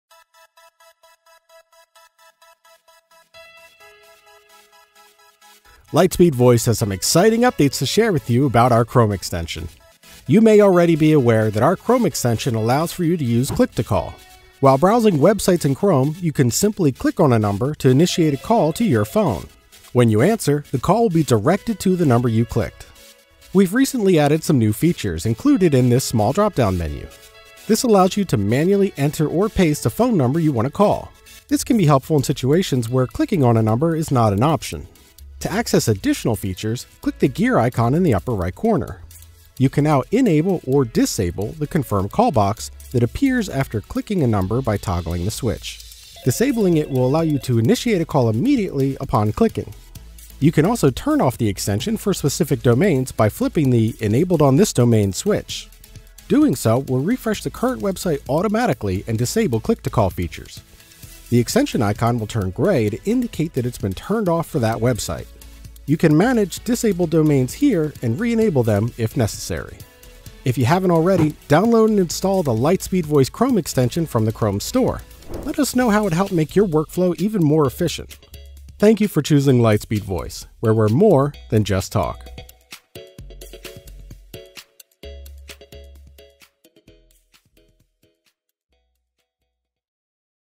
Lightspeed Voice's Chrome Extension: Enhancing Click2Call and Beyond
Lightspeed Voice's Chrome Extension allows users to leverage the convenient click2call feature while browsing websites on Google Chrome. By simply clicking on a phone number, you can initiate a call to your phone, and upon answering, the call will be connected to the number you clicked. It's that easy and efficient!

But wait, there's more! Lightspeed Voice has recently introduced an exciting new addition to the extension: a handy dropdown menu. With this new feature, you can manually enter or paste a phone number to make a call, which proves incredibly useful when clicking on a number is not an option.

Improved Flexibility: The new dropdown menu in the Chrome Extension allows users to manually enter phone numbers, expanding the use cases beyond click2call.

Customizable Settings: Access additional features by clicking the gear icon, such as enabling or disabling the "Confirm Calls" box. This feature lets you decide whether you want immediate call initiation or prefer an additional confirmation step.

Domain-Specific Control: You can now turn off the extension for specific domains. This granular control ensures that you can manage click2call features based on your needs for different websites.

Step-by-Step Guide:
Download and Install: If you haven't already, head over to the Chrome Store and download the Lightspeed Voice Chrome Extension.

Initiating Click2Call: Simply browse websites on Chrome and click on any phone number you want to call.

Using the Dropdown Menu: If clicking on a number isn't possible, click the extension icon and use the dropdown menu to manually enter or paste the phone number you wish to call.

Customizing Features: Access advanced options by clicking the gear icon. You can enable or disable the "Confirm Calls" box to adjust the call initiation process according to your preference.

Domain-Specific Control: To turn off the extension for specific domains, flip the "Enabled on this domain" switch. The extension icon will turn gray to indicate its deactivation on that website.

Remember, Lightspeed Voice's Chrome Extension is all about making your communication experience smoother and more efficient.

00:00 Intro
00:12 Demonstration
00:24 Dropdown Menu
00:39 Confirm Calls
00:50 Disable Domain
01:04 Manage Disabled Domains
01:14 Chrome Store